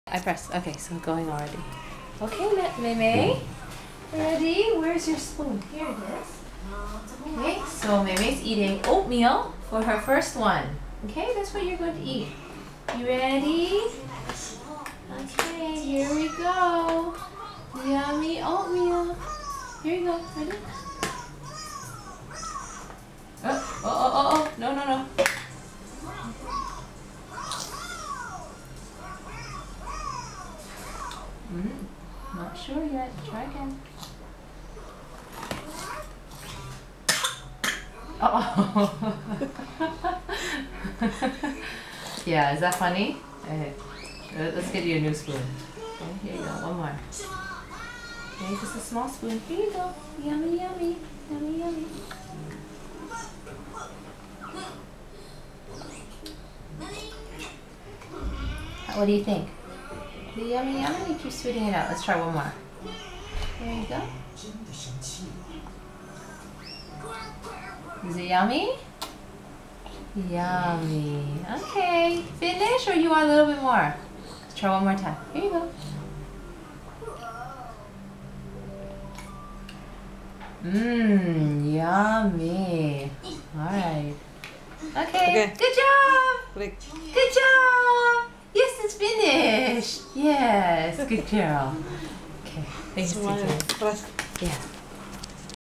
Baby Lauren eating solids for the first time
She does a great job opening her mouth and eating her first few bites of solid food.   Yummy!